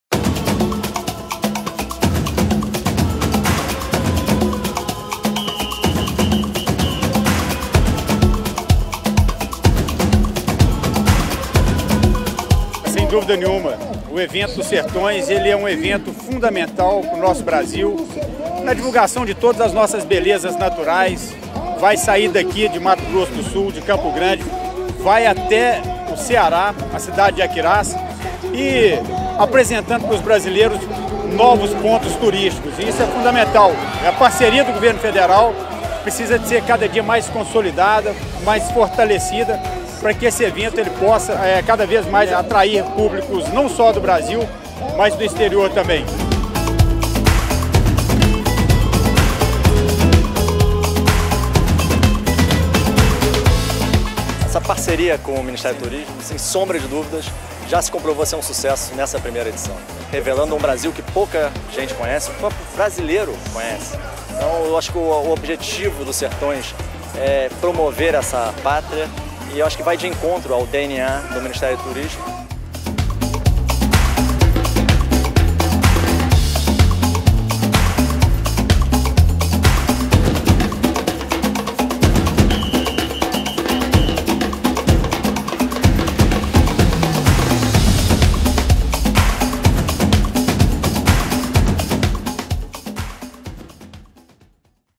MTurNoSertões - Sobe a poeira em Campo Grande
O coração dispara quando lembramos da emoção que vivemos nos mais de 4.700 km pelo interior do país com os Sertões! Vamos mostrar, bem rapidinho, as belezas naturais do trajeto entre o Mato Grosso do Sul e o Ceará!